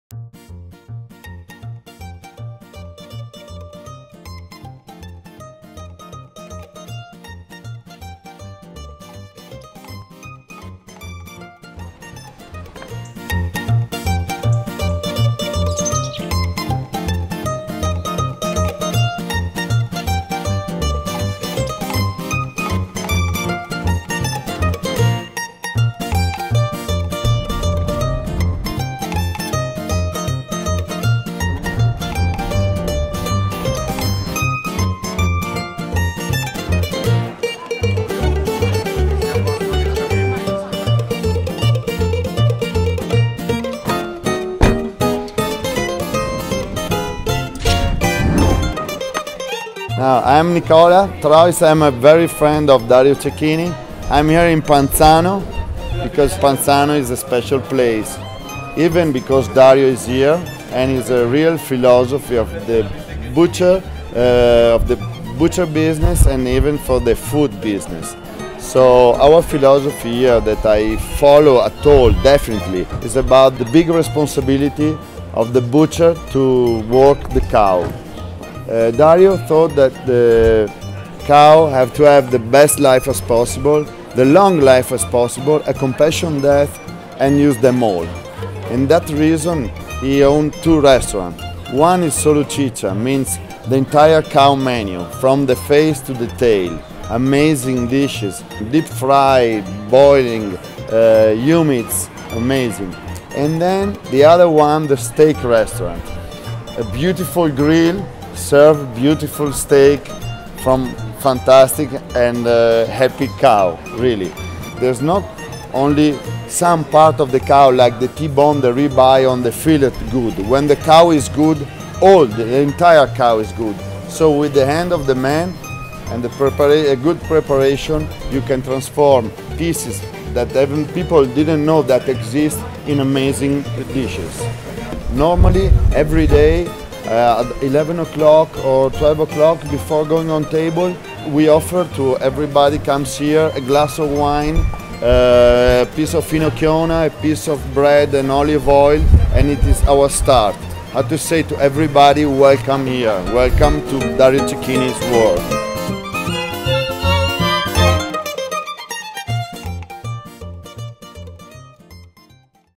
Visa Signature concierge service for Pavel Lungin (from Tuscany) /Dario Cecchini/Panzano
Специально для премьеры карты Visa Signature, мы спросили известных людей,  чего им больше всего не хватает в Москве...И осуществили их желания с помощью консьерж службы карты.